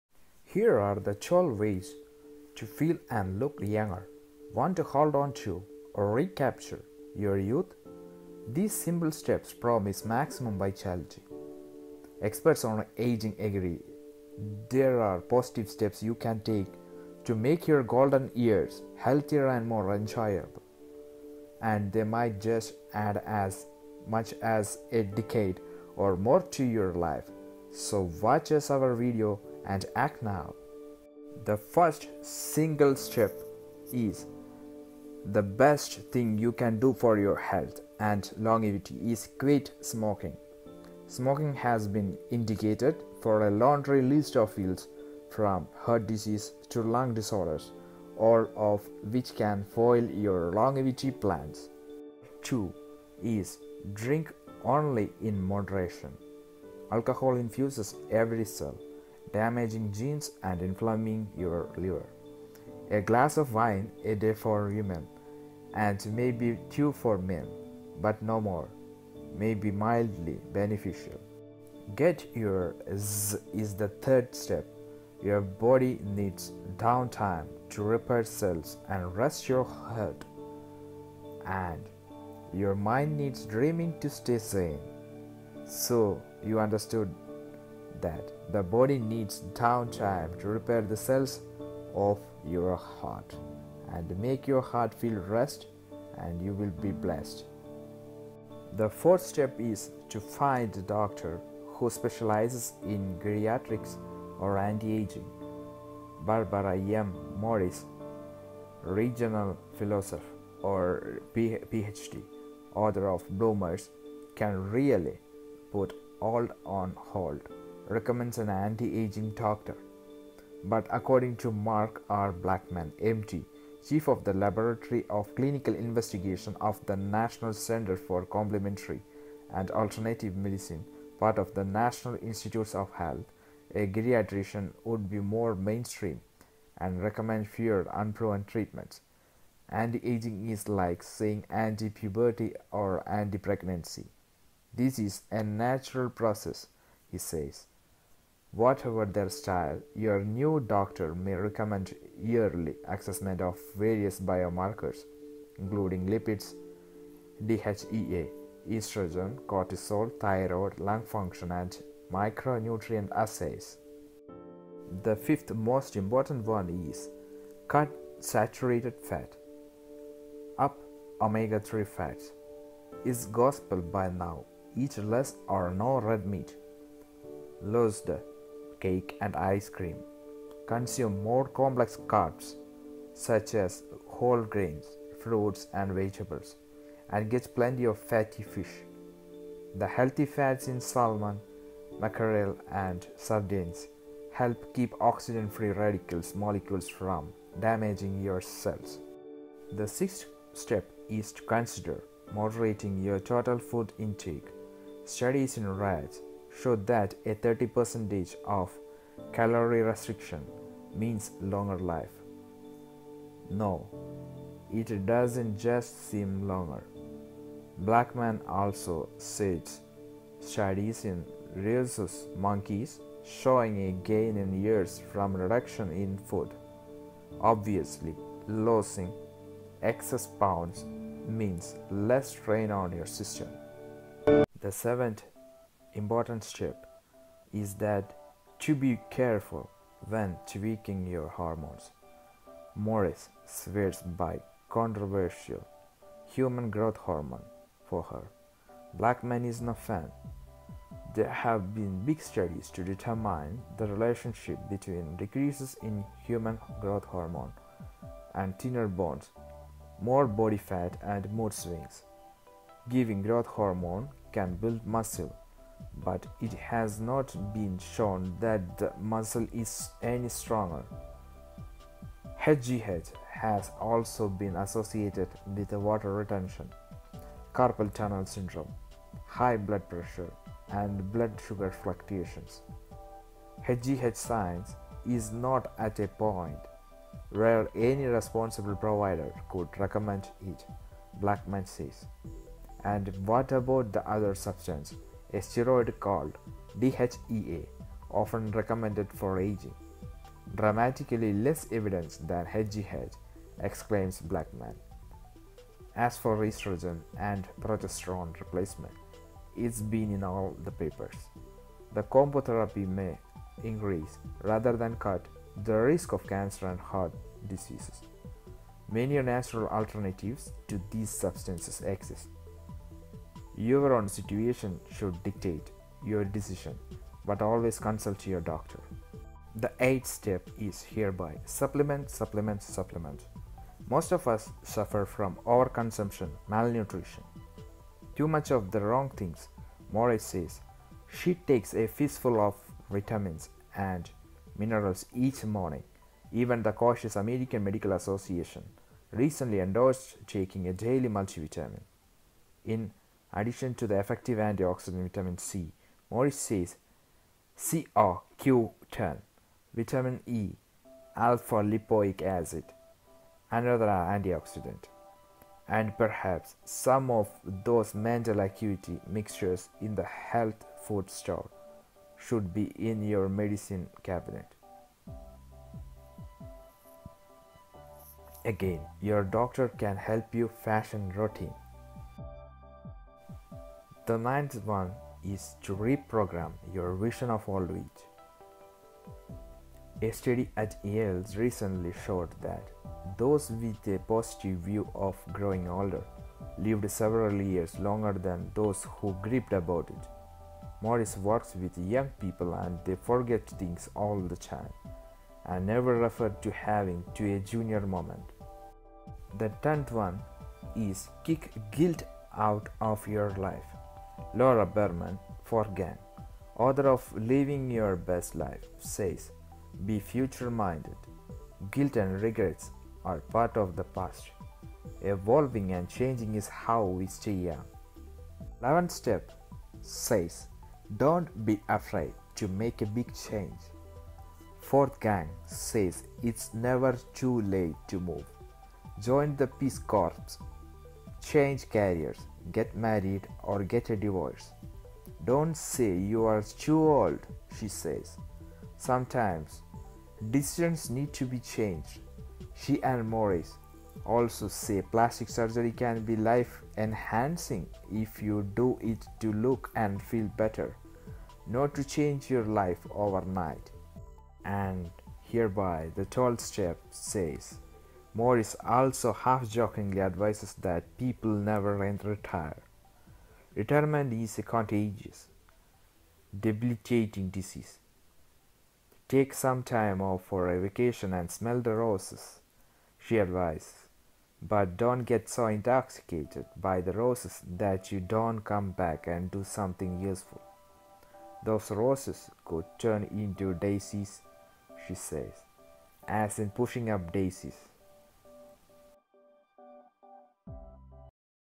12 steps which keeps you young !!!
Want to hold on to -- or recapture -- your youth? These simple steps promise maximum vitality. Experts on aging agree -- there are positive steps you can take to make your "golden years" healthier and more enjoyable. And, they might just add as much as a decade or more to your life...